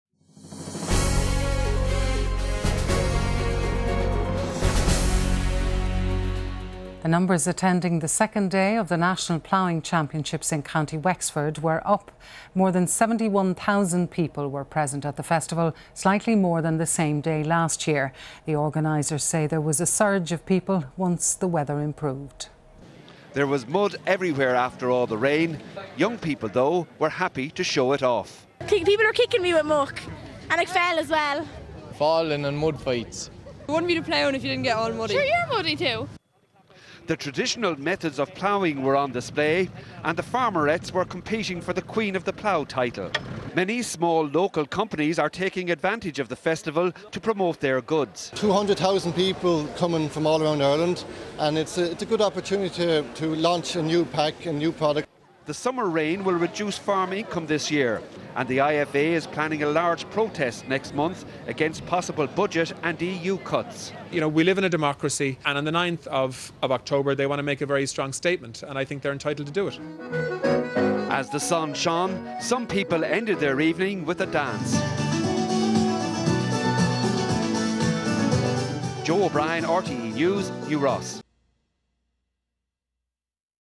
Day 2 of the Ploughing Championships
The sun came out for day 2 of the Ploughing Championships in New Ross